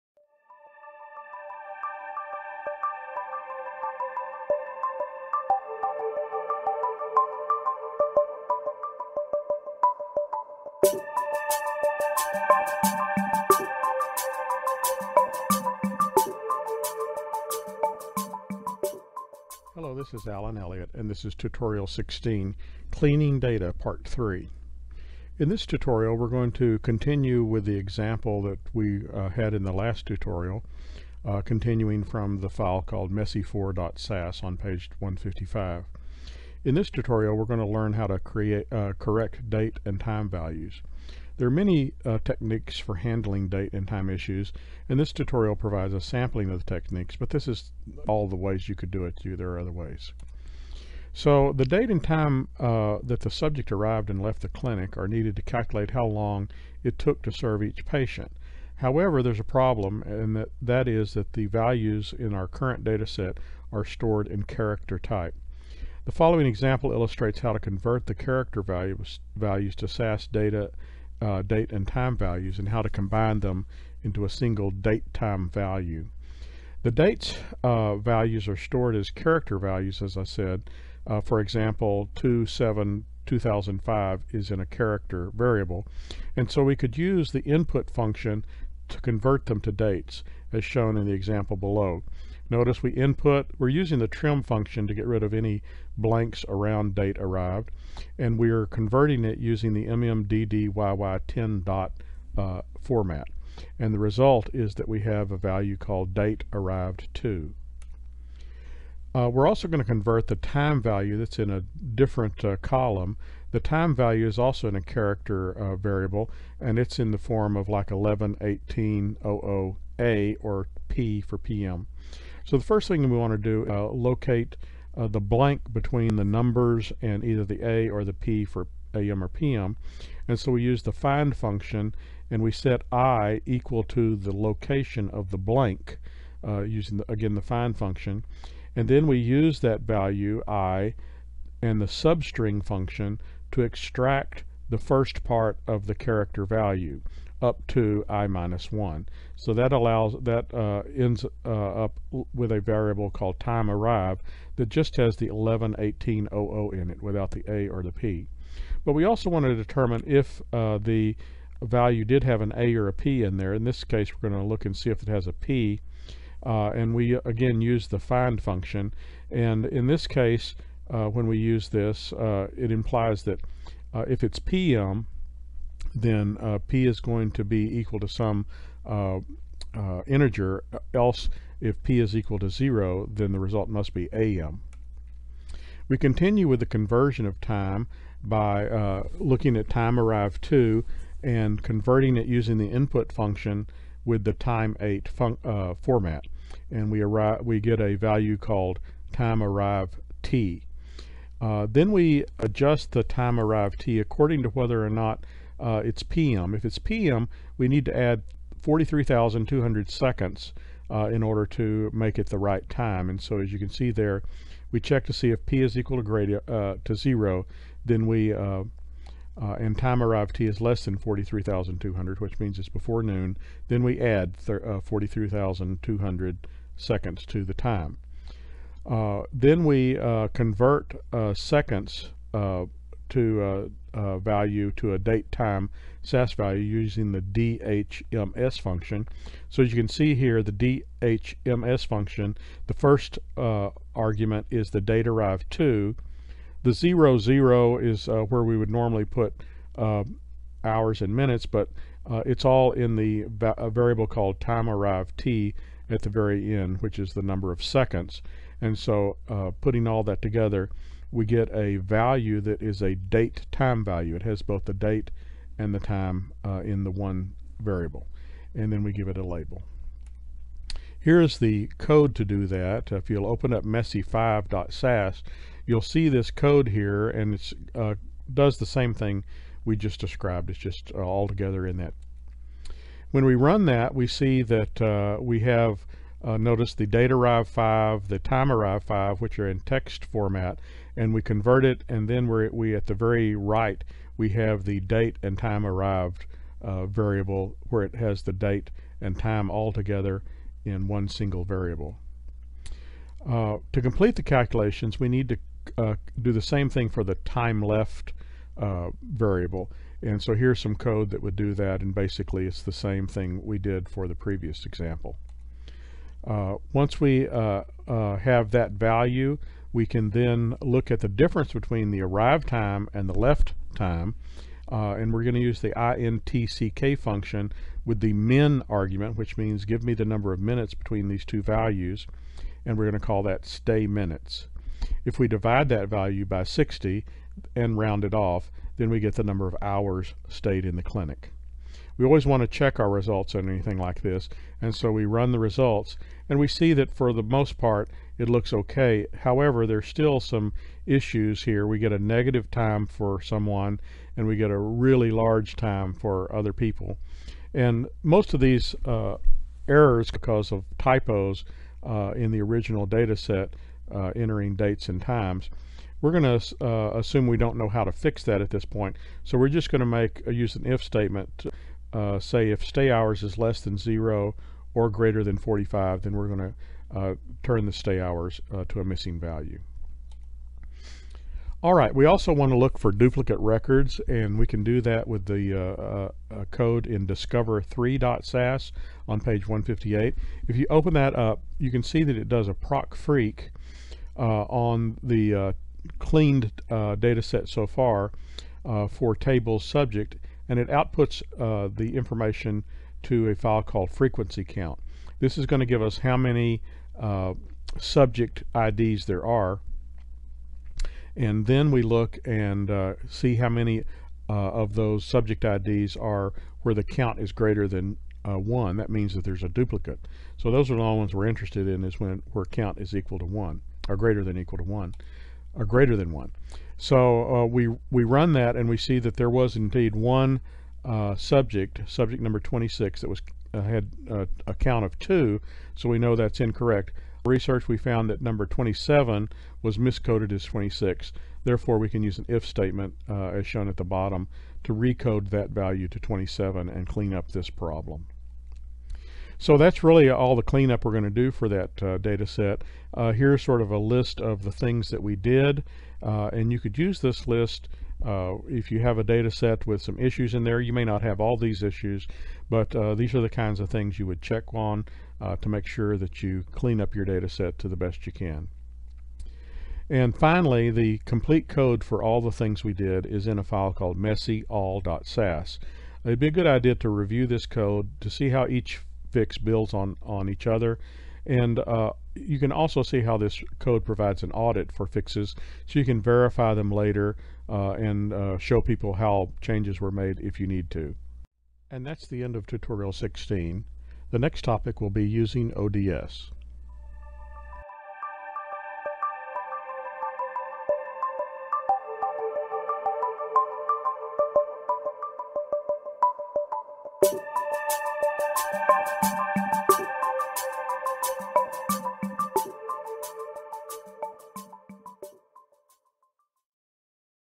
SAS Tutorial Basics 16 DATA STEP - Dealing with Messy DATA sets part 3
SAS Tutorial for Beginners 16 DATA STEP - Part 3 of 3 - Dealing with Messy DATA sets -- From SAS ESSENTIALS by Elliott and Woodward, 2nd Edition. SAS DATA STEP Tutorial 16 Messy DATA 3 by Alan Elliott on cleaning a messy data set using SAS Software. This gives yo a step-by-step description of how to clean up a data set and prepare it for analysi